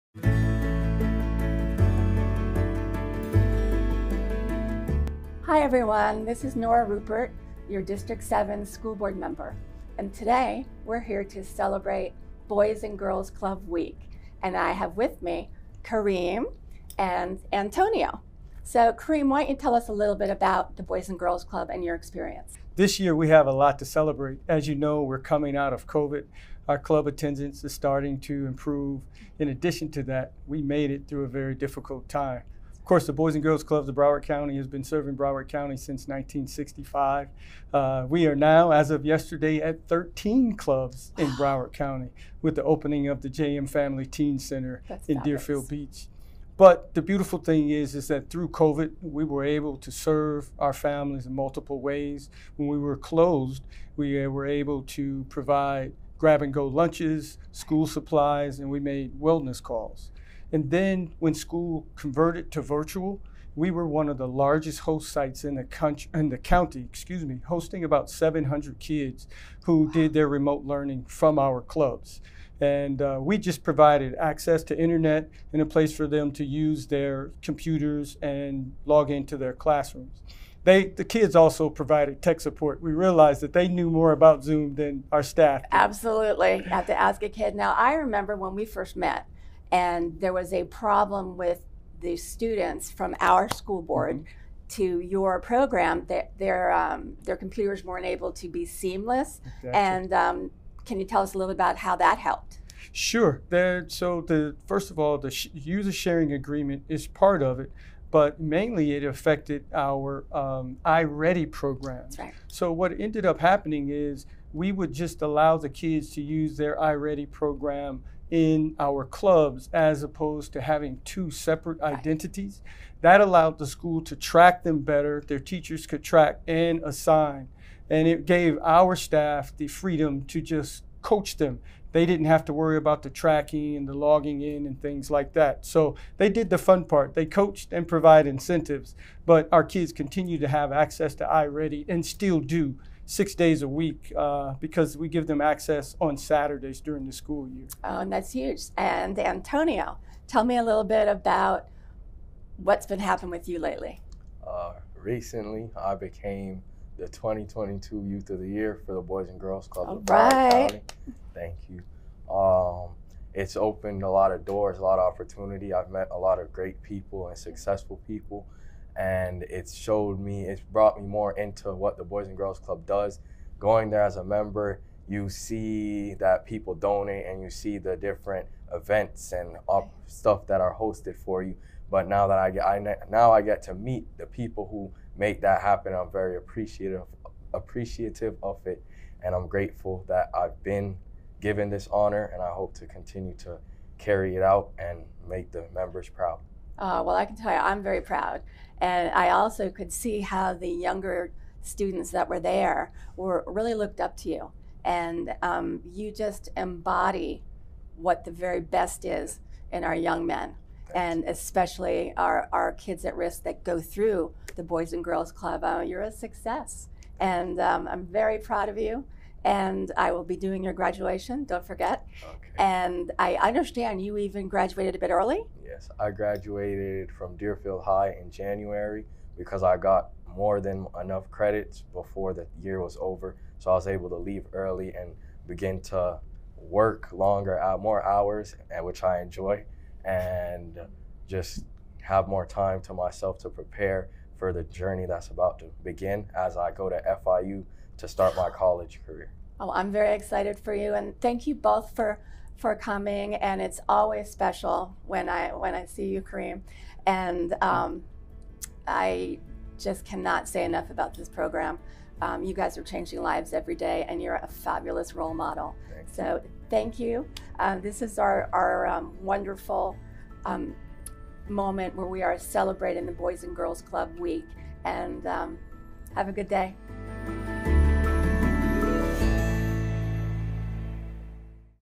BCPS Celebrates Boys and Girls Club Week, June 25 – July 1
BCPS Celebrates Boys and Girls Club Week, June 25 – July 1! Watch a great conversation with School Board Member Nora Rupert, and the Boys and Girls Club of Broward County VP of Development and its Student of the Year.